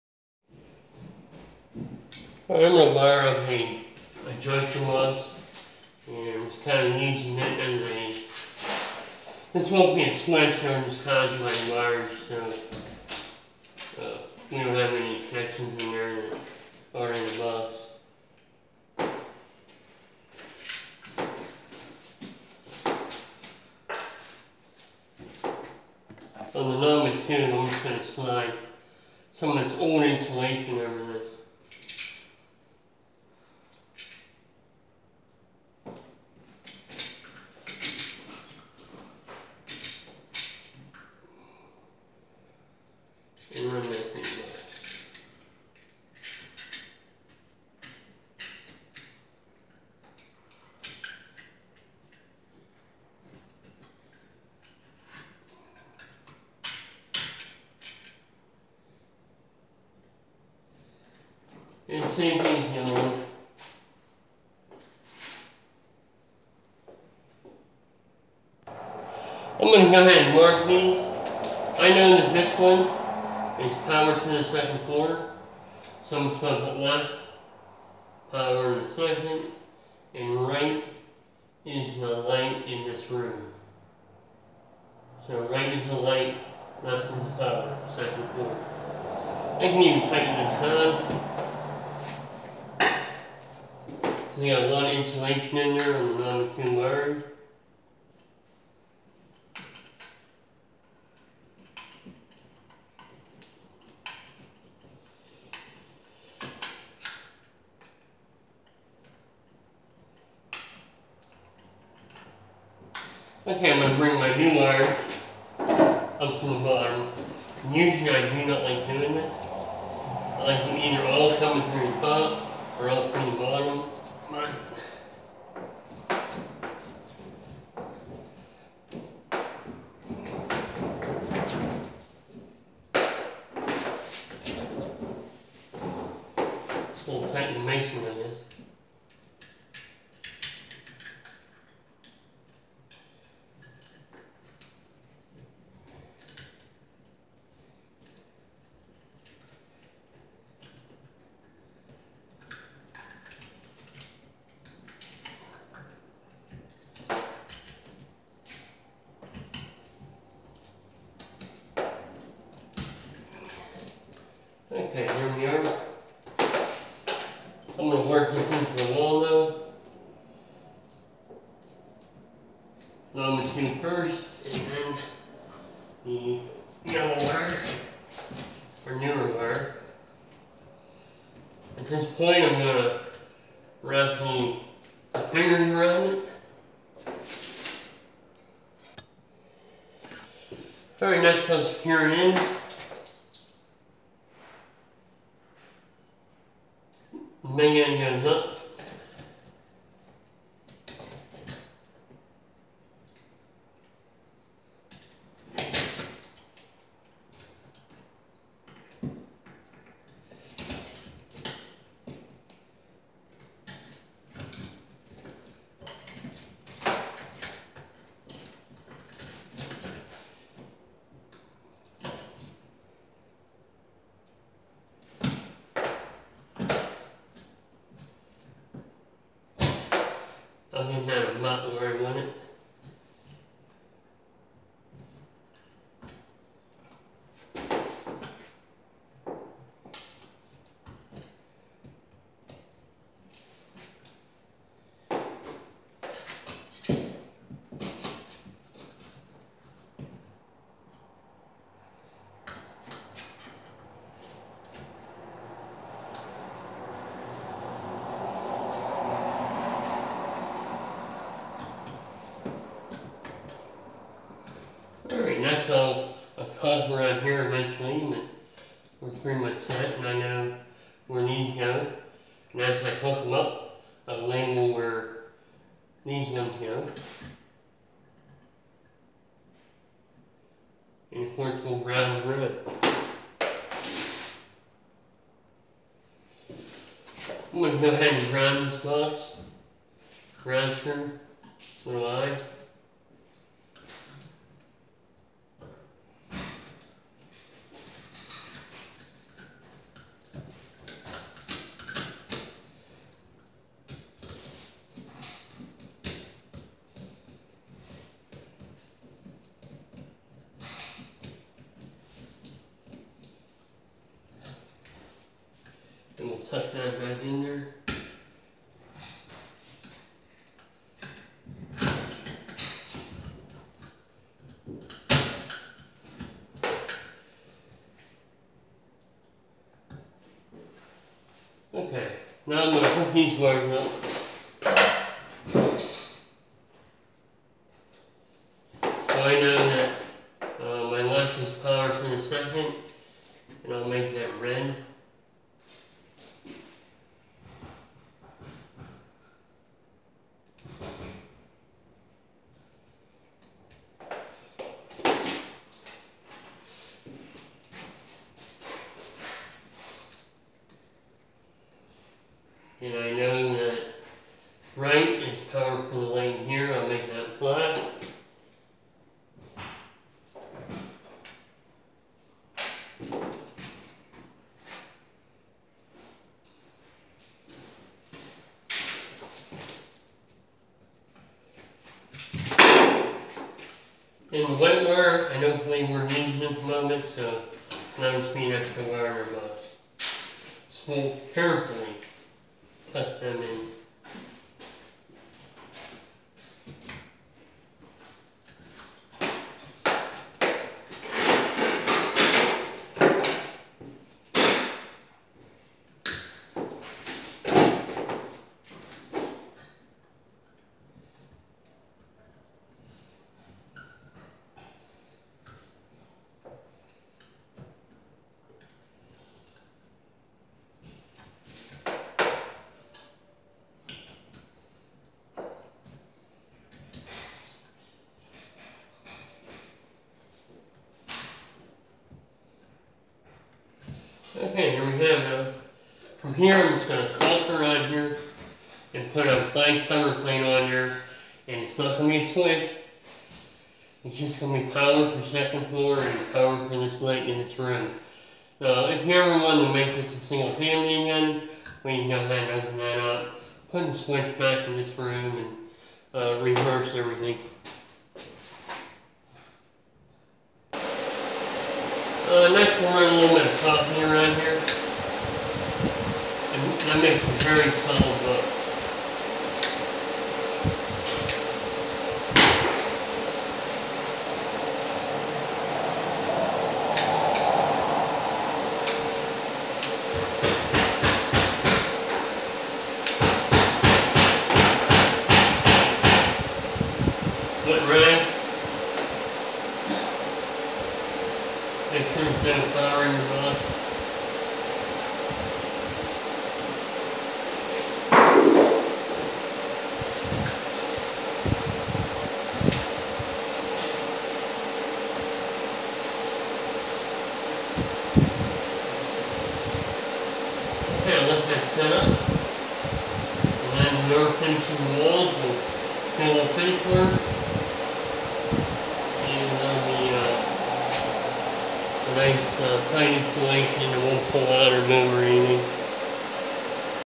Wiring electric box includes Knob and tube
Using an old electric box to wire circuits on the first floor and the second floor. This old work box will serve as a junction box. We will wire K&T, BX and romex connections.
Amazon link to Great products that every DIYer and Handyman should know about.